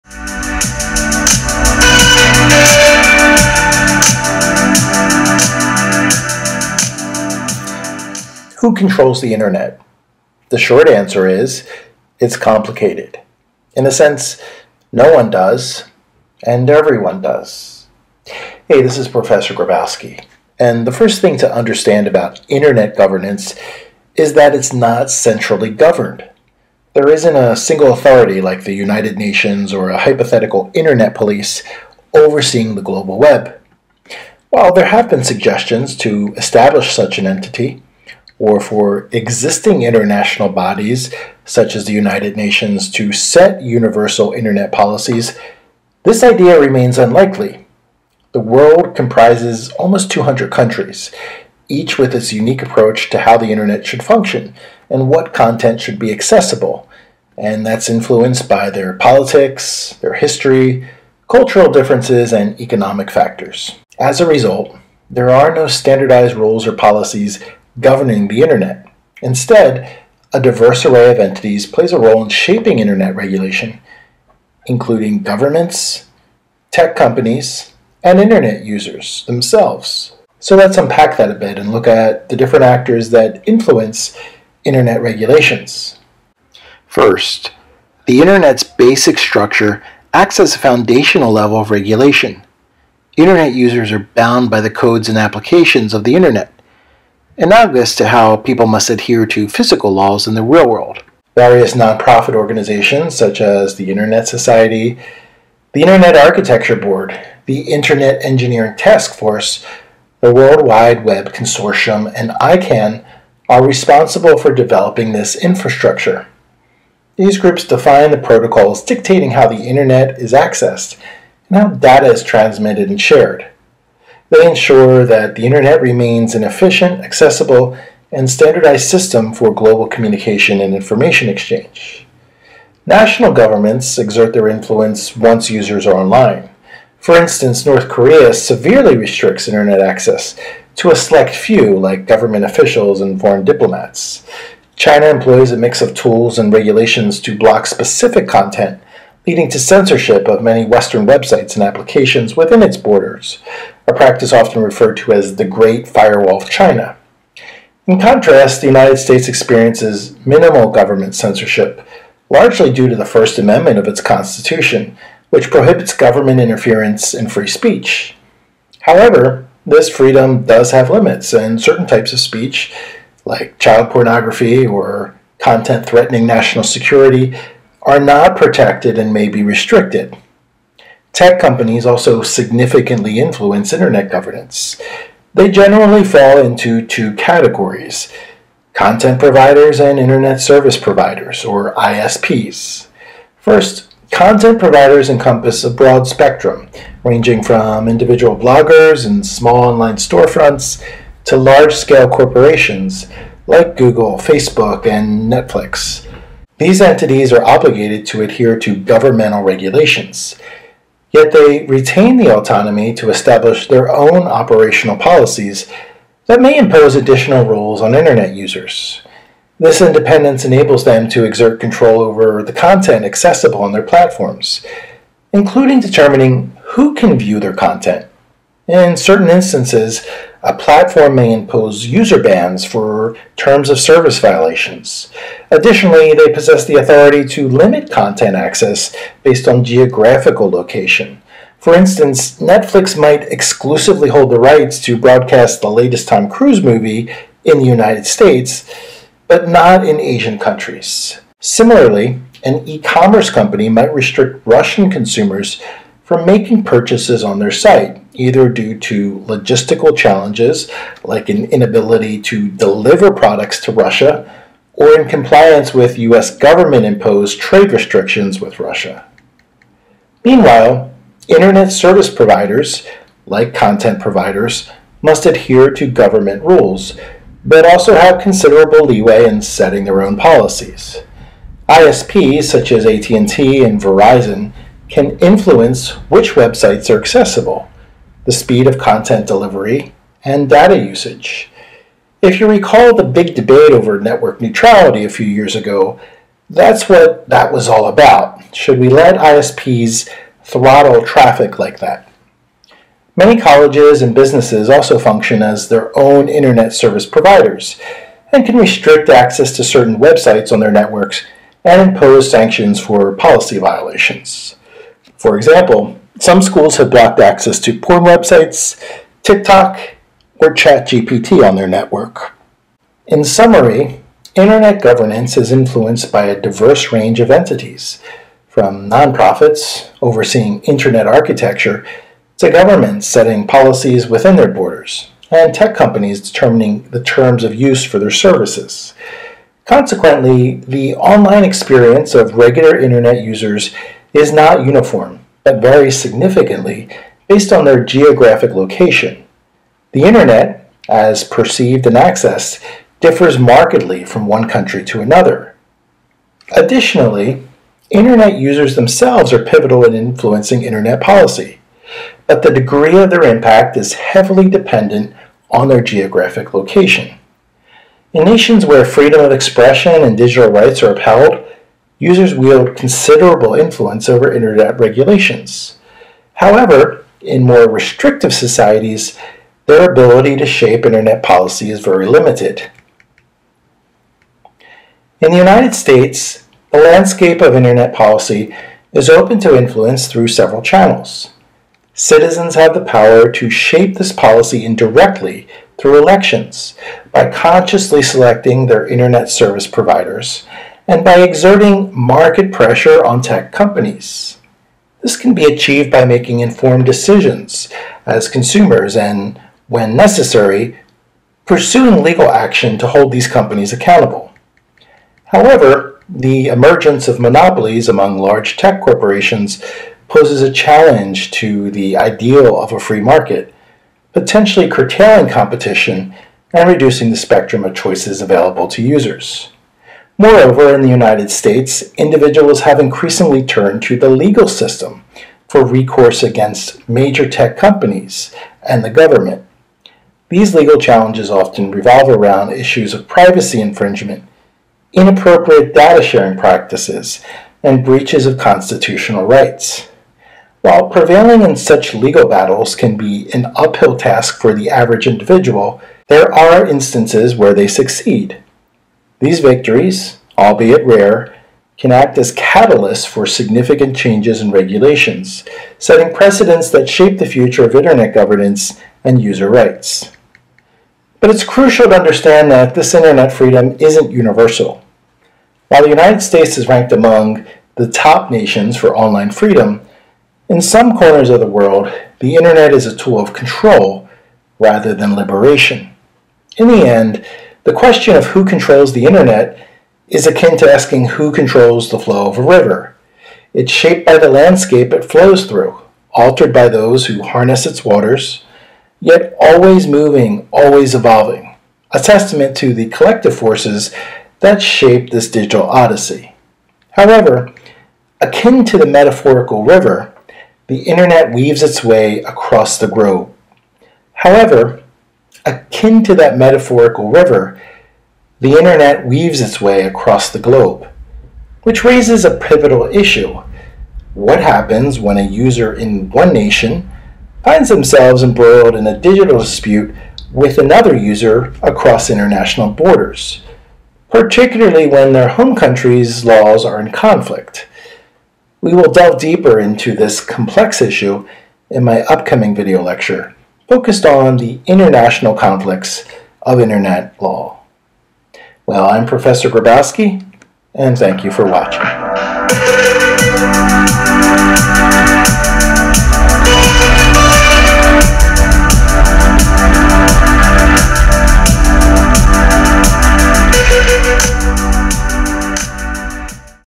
Internet Governance: Who Controls the Internet?
Who controls the Internet? This 10-minute lecture by cyber law professor Mark Grabowski examines how governments, content providers, ISPs and internet users all shape internet policy and regulations.